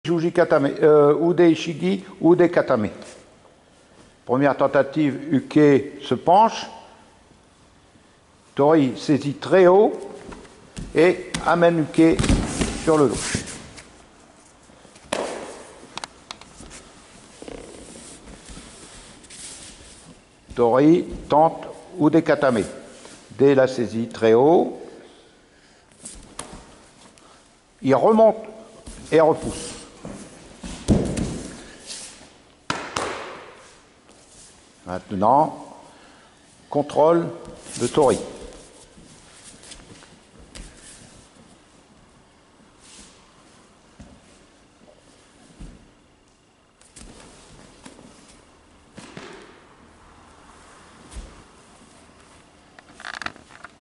15 UDE HISHIGI UDE GATAME dégagement de Uke et contrôle de Tori
Le KATAME NO KATA en étude au Dojo d'Etaples sur mer ce dimanche 22 janvier 2023. Technique par technique avec explications et situations de sorties de UKE et de  contrôle final de TORI